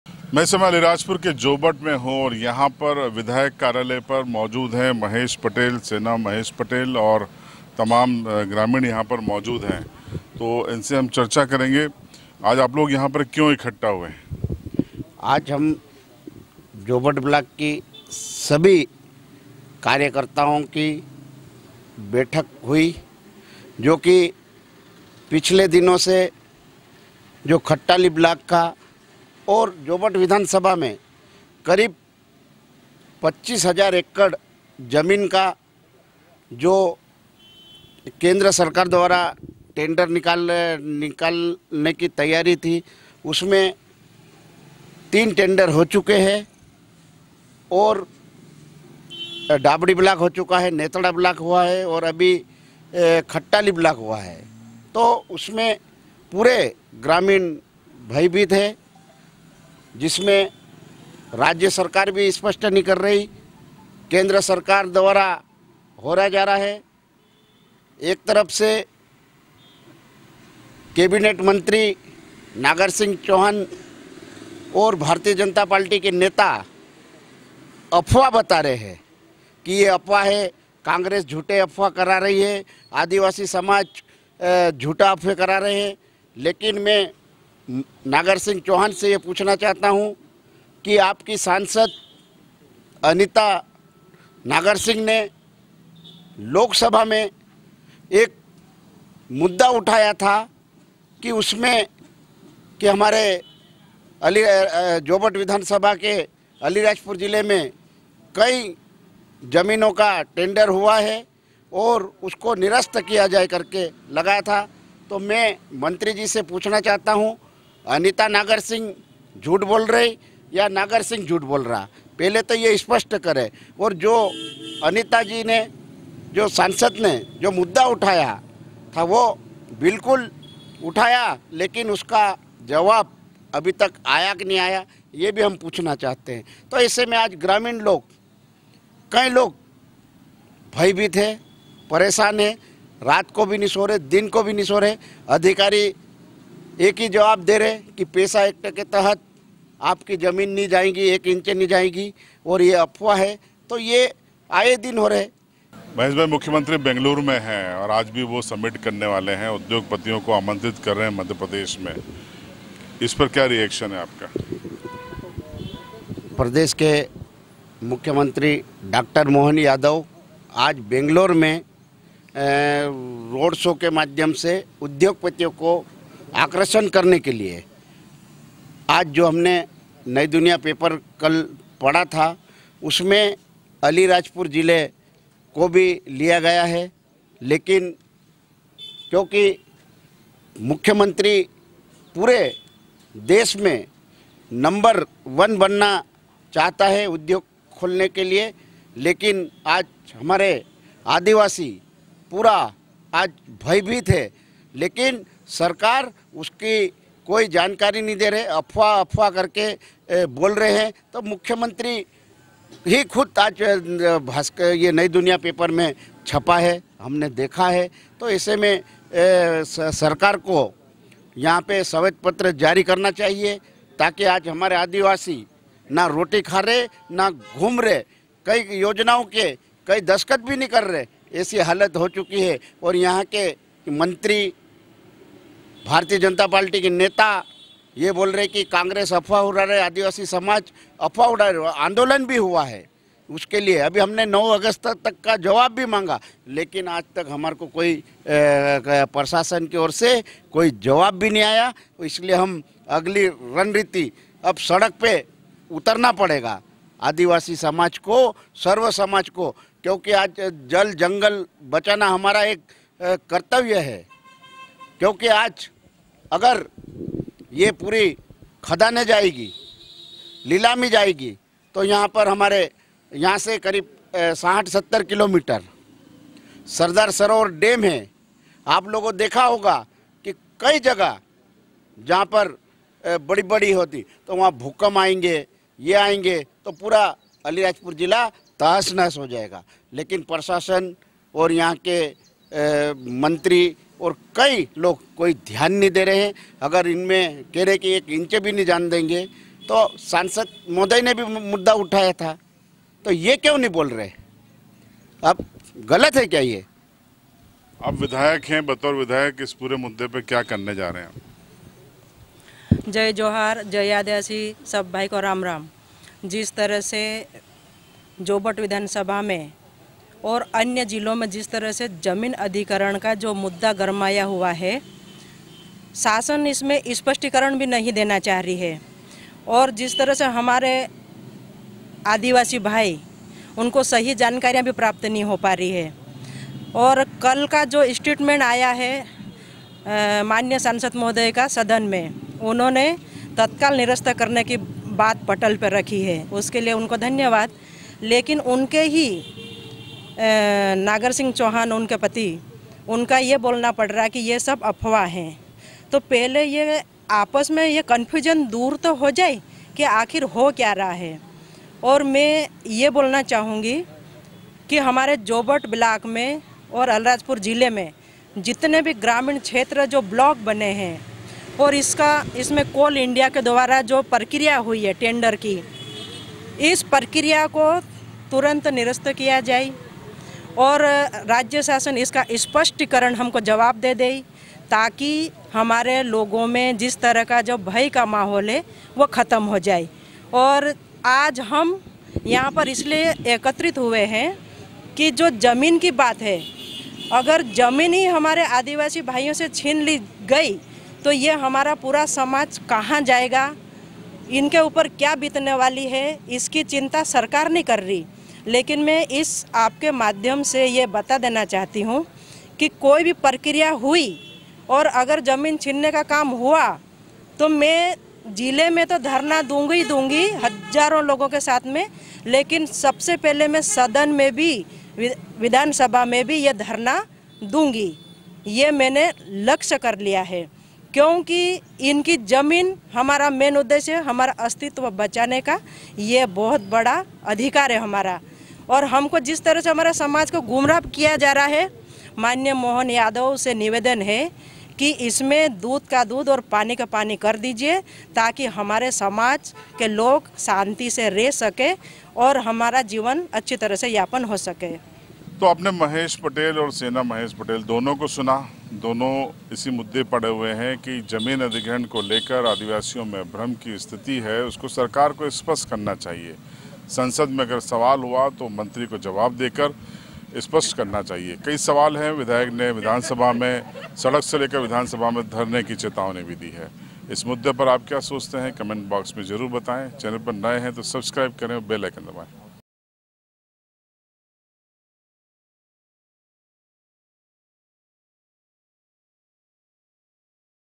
खट्टाली ग्रेफाइट ब्लाक मामले में कंफ्यूजन जारी , महेश पटेल ओर सेना पटेल के नये आरोप
जोबट मे कांग्रेस विधायक सेना महेश पटेल ने लगाया जनता दरबार

ग्रामीण क्षेत्र के लोगो की समस्याओ को सूना ओर किया तत्काल उसका निराकरण

आलीराजपुर ÷ अलीराजपुर जिले की जोबट विधानसभा क्षेत्र की कांग्रेस विधायक सेना महेश पटेल ने आज गुरूवार को विधायक कार्यालय पर जनता दरबार लगाया जहाँ उन्होने ग्रामीण क्षेत्र से आए कार्यकर्ताओ ओर लोगो की समस्याओ को सूना ओर उसका निराकरण किया विधायक सेना पटेल ने कहा के मेरी विधानसभा क्षेत्र के लोगो की जो भी समस्या हो उसको विधायक कार्यालय पर आकर मूझे या मेरे किसी भी कार्यकर्ता को उस समस्या से अवगत कराये तत्काल उसका निराकरण किया जाएगा आज जोबट ब्लाक के कई ग्रामीण लोग बिजली पानी ओर अन्य समस्याओ को लेकर विधायक के पास पहूचे थे वही खटटाली ग्राम के क ई ग्रामीण जो कोल ब्लाक के लिए जो जमीन अधिग्रहण की है कोल इंडिया कम्पनी के द्वारा उसे निरस्त कराने की मांग को लेकर भी क ई सरपंच ओर ग्रामीण विधायक के पास पहूचे थे विधायक ने कहा के वो विधानसभा मे इस मामले को उठाएंगी ओर प्रदेश की मूख्यमंत्री मोहन यादव को भी इस बात से अवगत करायेगी की आदिवासीयो की जमीन का अधिग्रहण नही किया जाए इस दोरान कांग्रेस के डाक्टर आराम पटेल ,सरदार आज़नार,ब्लॉक अध्यक्ष कालू मेड़ा,अरविंद बबलू डावर,चेन सिंह डावर विधायक प्रतिनिधि मोनू बाबा सूनील खेडे जीतू अजनार सोनू वर्मा ,लक्की राठौड़,रफीक भाई सहीत  सरपंच जनपद सदस्य एवं अन्य कांग्रेस कार्यकर्ता मौजूद थे